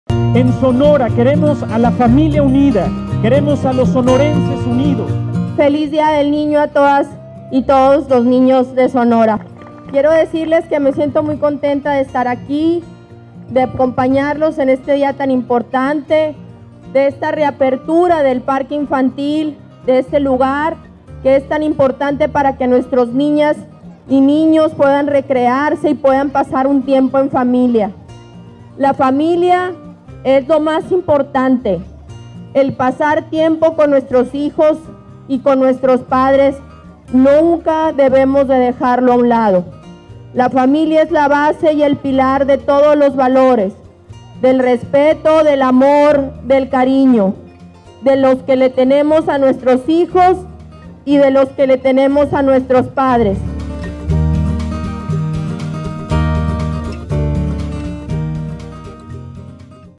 Vuelve el Parque Infantil
La Gobernadora Claudia Pavlovich asistió a la reapertura del Parque Infantil de Hermosillo, para celebrar el día del niño en compañía de las familias sonorenses.